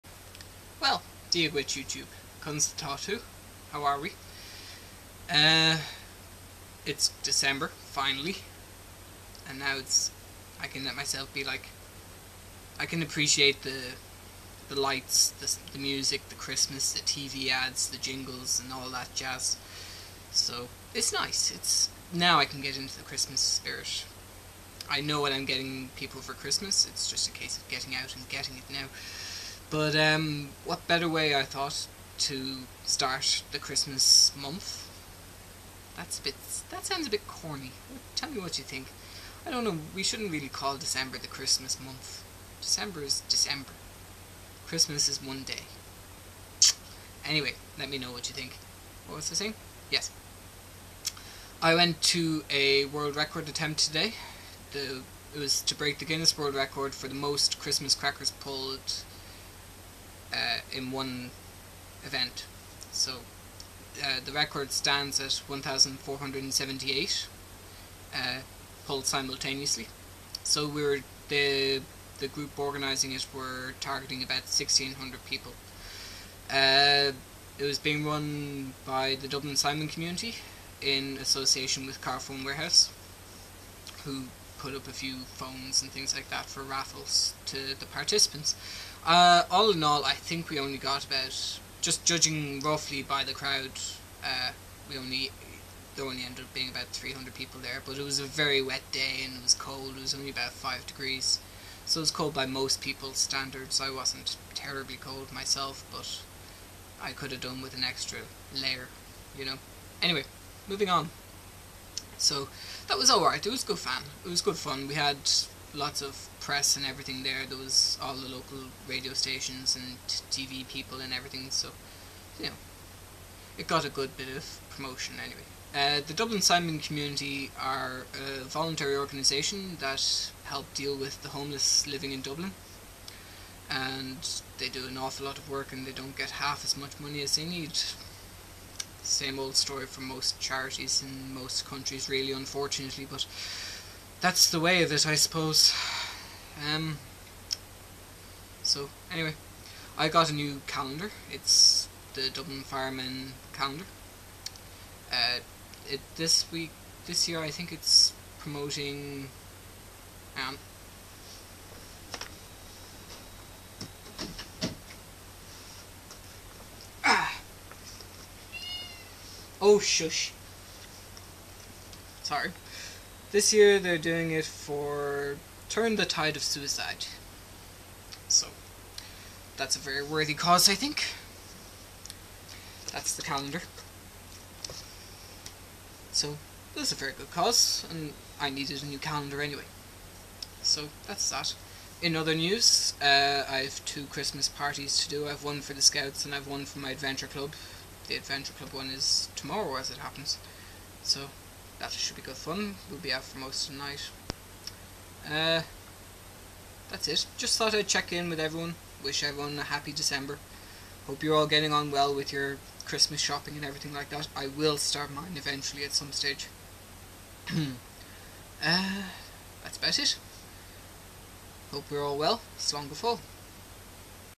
Wooo December!
Dia duit.
Well it is here at last, a fast year but a long one at the same time.
Got a bit wet and cold today but it was good fun.
Forgot to mention I had to go to the dentist first thing this morning, nothing but a routine check up thankfully and all is good :)

Hope all is going well for you;
Slán go fóill